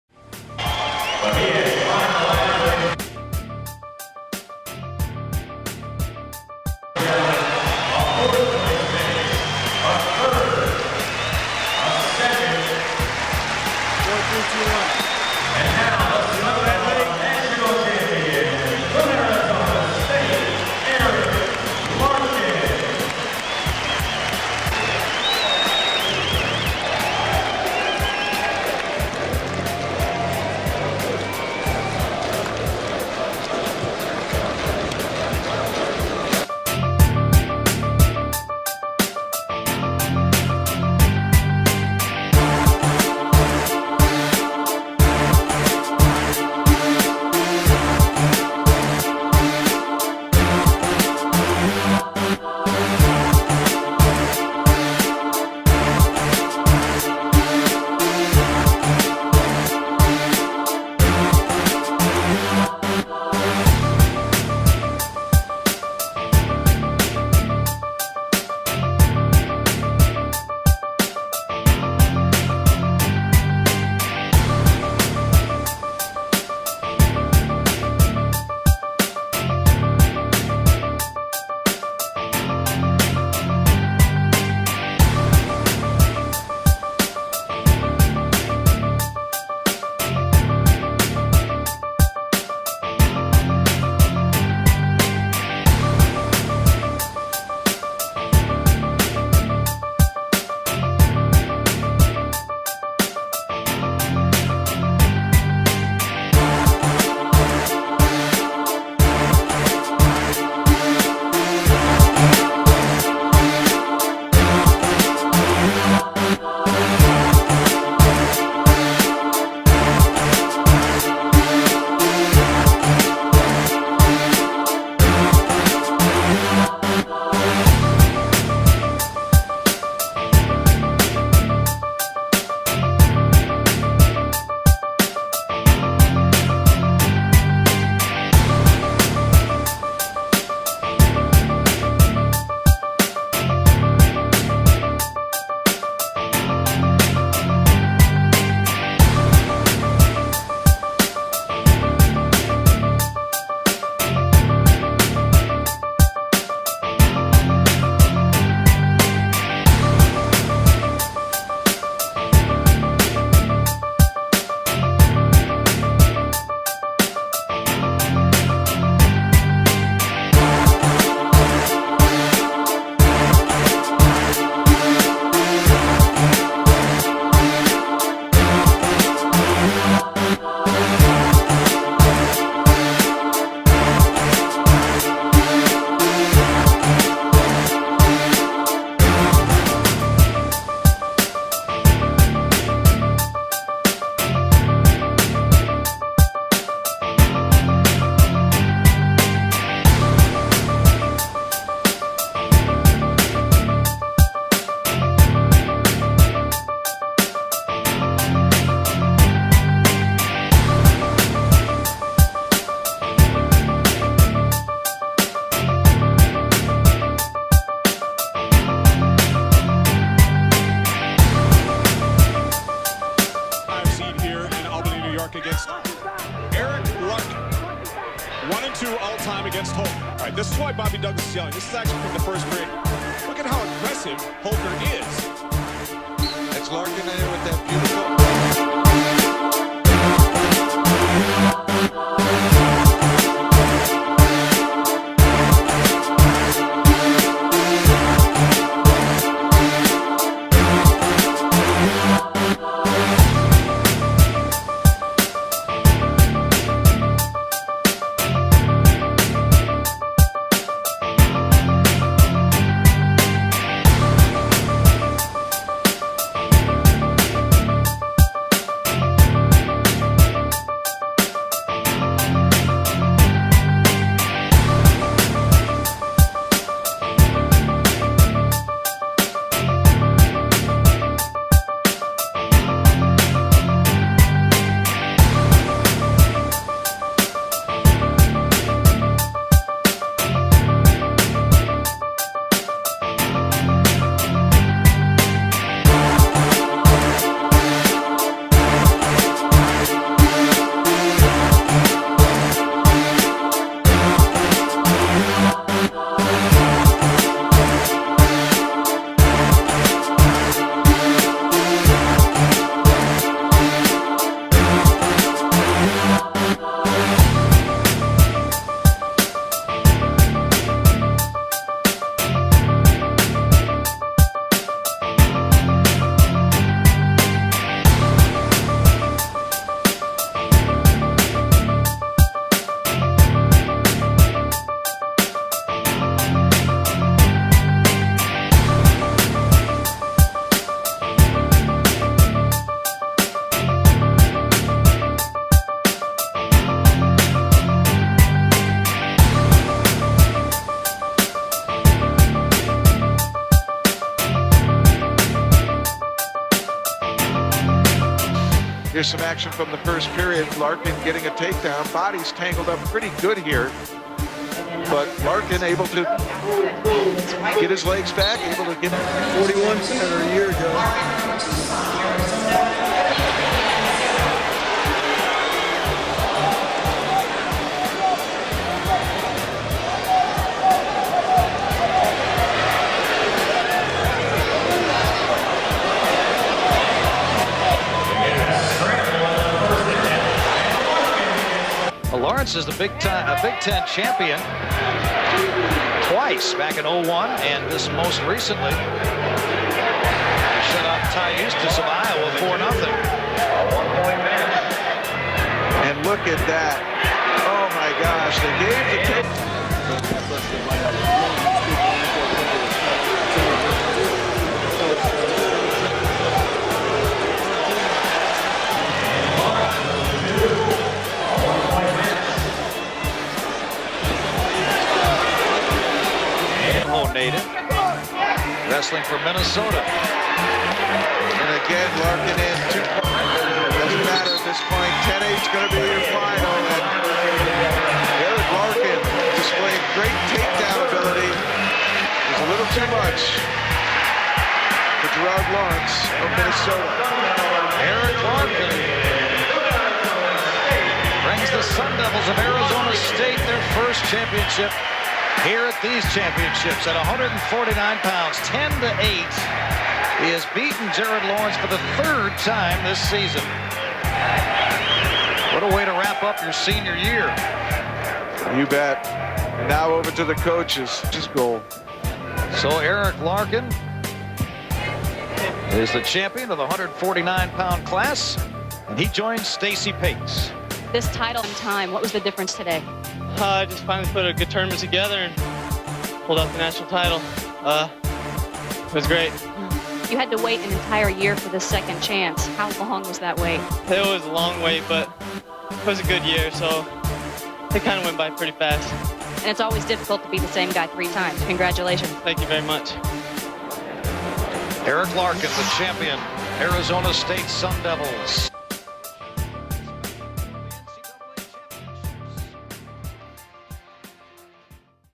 Eric Larkin Wrestling HL Video
This video is a hi-lite of Eric Larkin's wrestling career.